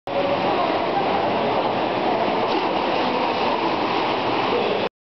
Réka ugrik
Réka bátran bombát mutat be